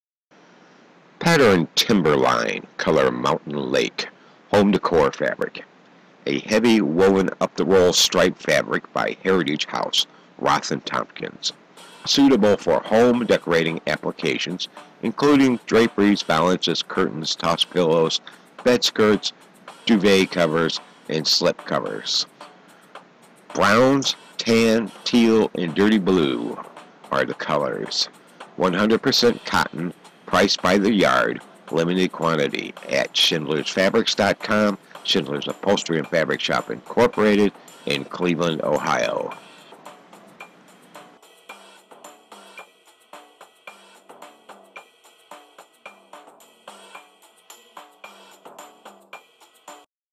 Pattern Timberline color Mountain Lake home decor fabric
heavy woven up the roll stripe fabric by Heritage House, Roth & Tompkins

suitable for home decorating applications, including draperies, valances, curtains, toss pillows, bed skirts, duvet covers, and slipcovers

Browns, tan, teal and dirty blue

repeat 13 1/2"H 54"W

100% cotton

price by the yard, limited quantity